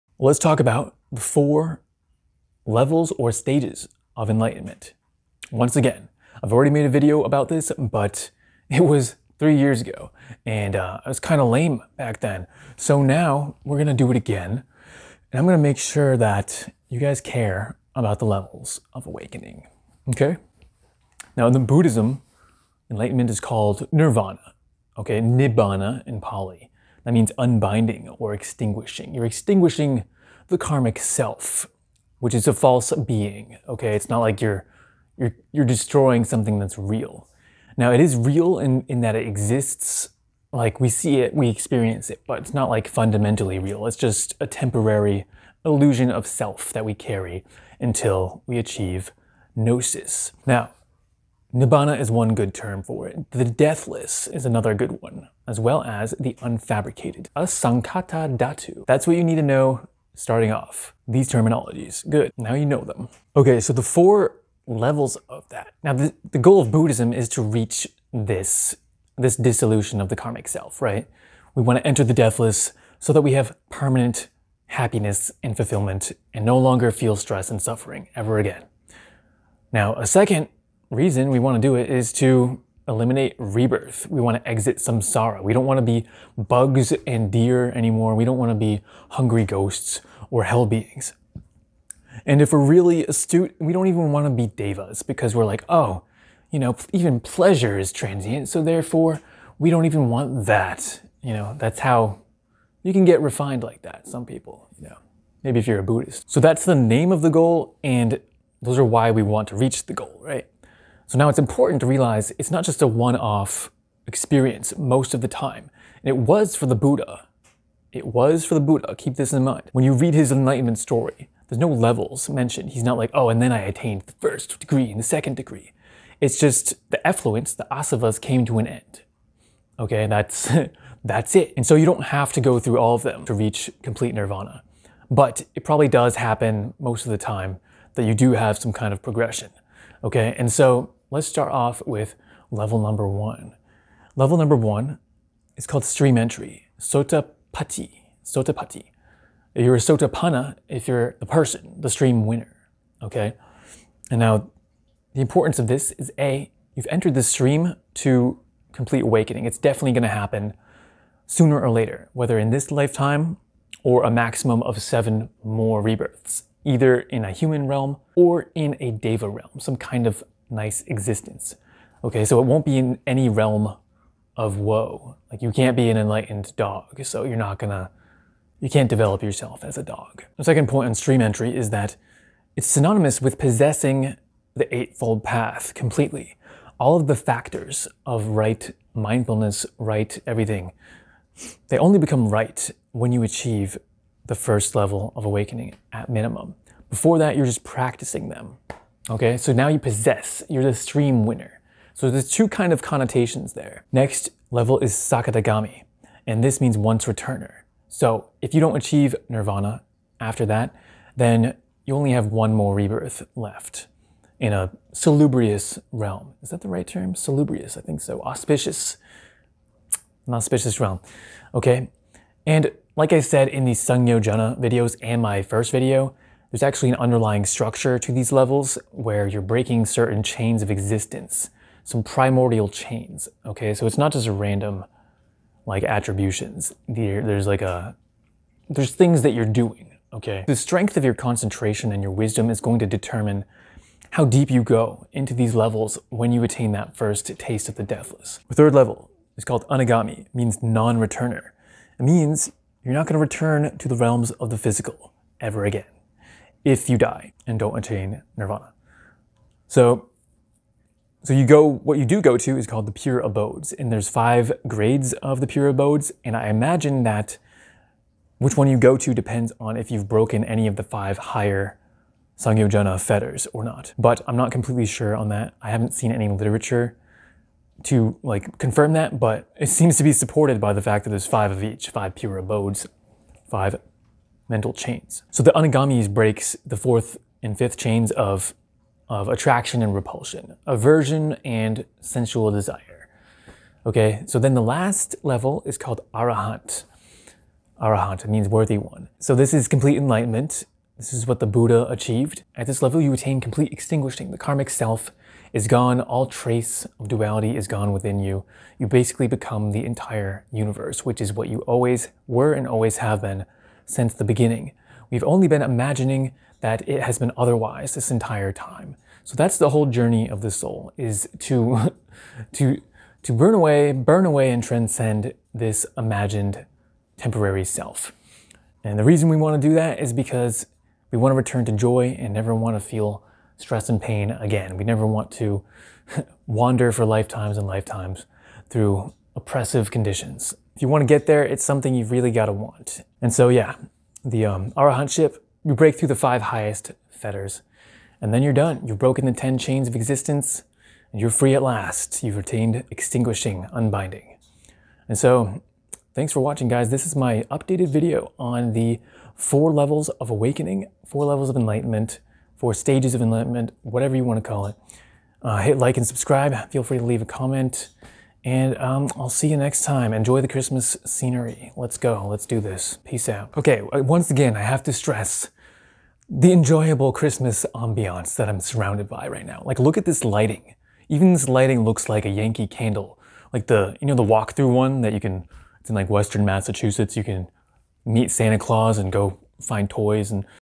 Revisiting the 4 Levels of Enlightenment || Theravada Buddhism
This is my updated video speaking about the 4 stages of Awakening. The previous one is more regimented and also focuses on the meaning of the 10 samyojana, so this time I present it in more of an informal vlog format.